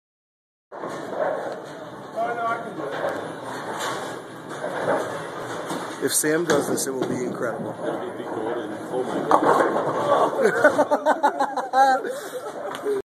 Goldberg goes for the spare
Sam went for 4/7 + 10 split. Little over confident w/the victory dance before the pins fall, but it's the spirit that counts! sam is rocking his new ball in this vid.